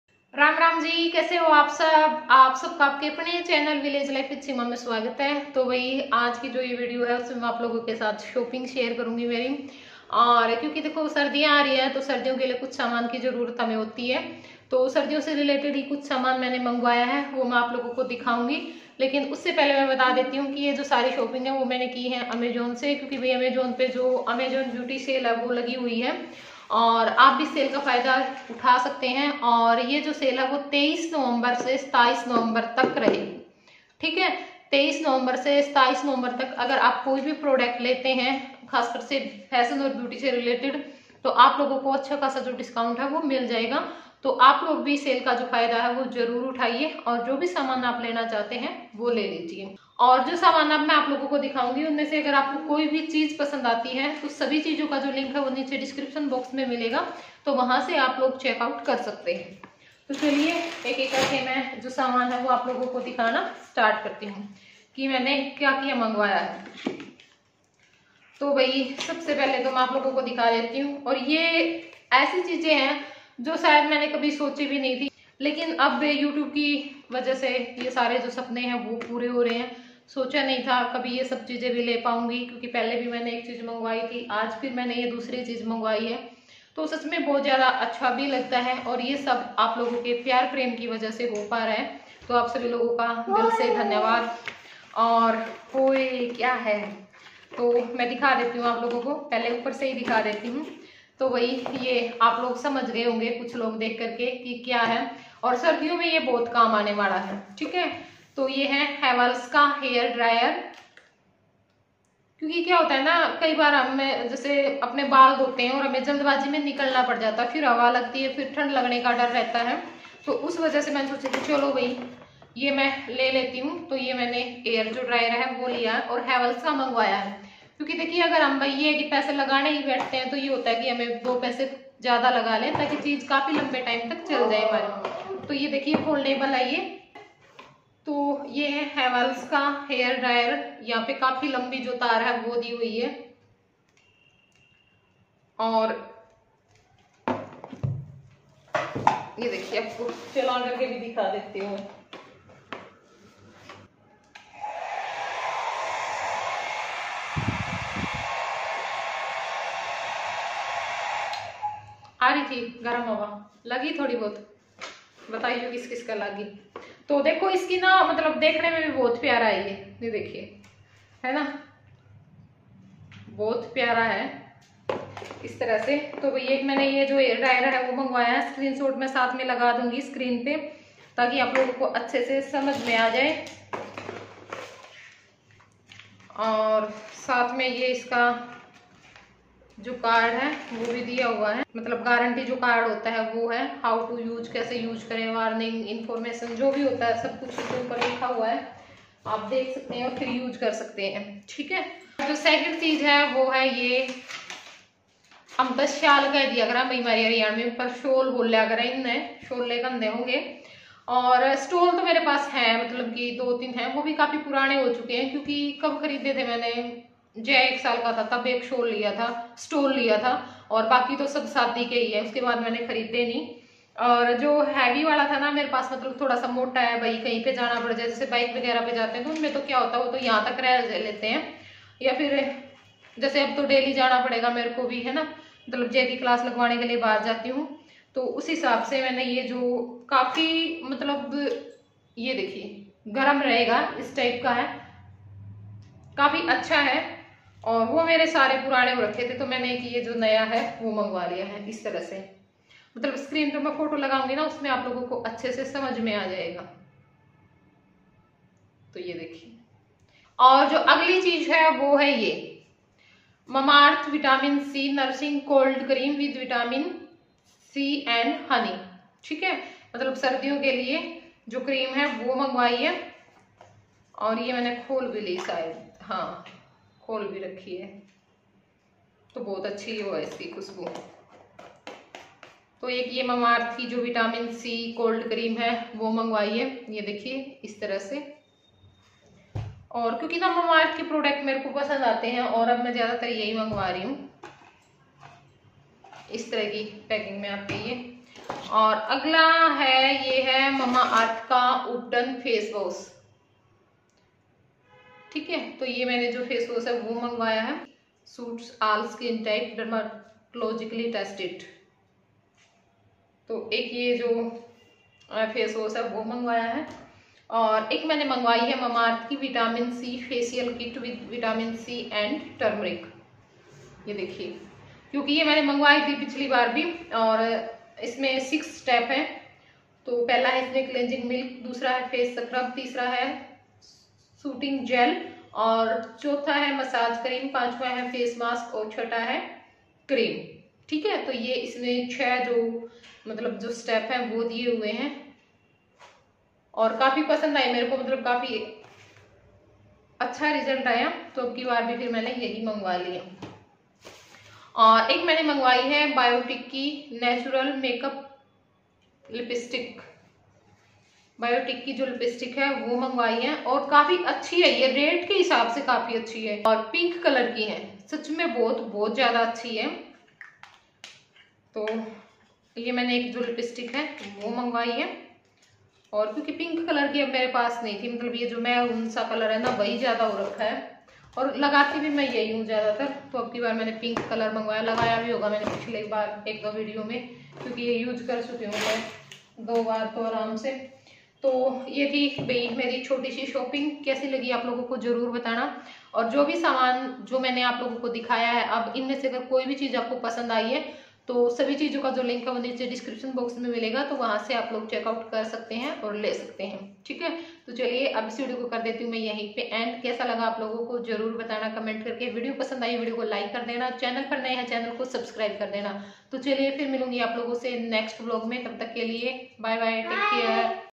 Amazon’s The Beauty Sales 23-27Nov. यूट्यूब के कारण सपने पूरे हो रहे है🤗कभी ये सब चीजें भी ले पाउंगी
Amazon’s The Beauty Sale
sale Dates: from 23rd Nov to 27th Nov

Buying Links :
1.  Mamaearth Ubtan Natural Face Wash for All Skin Type with Turmeric & Saffron for Tan removal and Skin brightning 100 ml - SLS & Paraben Free
2.  Weavers Villa Kullu Design Shawl (Coffee)
3.  Mamaearth Vitamin C Facial Kit with Vitamin C & Turmeric for Skin Illumination - 60 g
4.  Mamaearth Vitamin C Nourishing Cold Winter Cream for Face & Body with Vitamin C & Honey for Illuminating Moisturization - 200g
5.  Biotique Natural Makeup Magicolor Lipstick, Winter Kiss